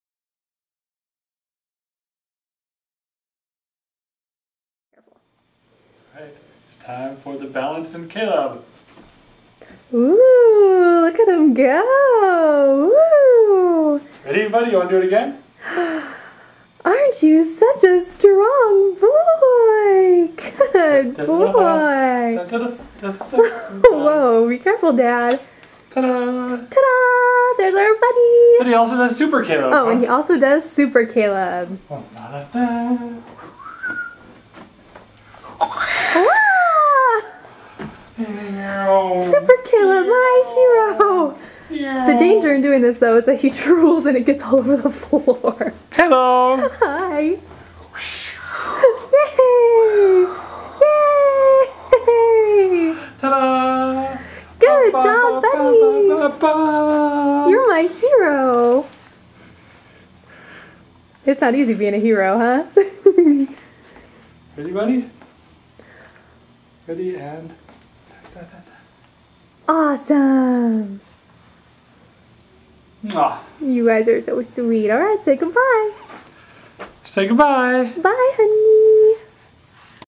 The amazing Caleb!
The amazing Caleb flies and balances with the greatest of ease.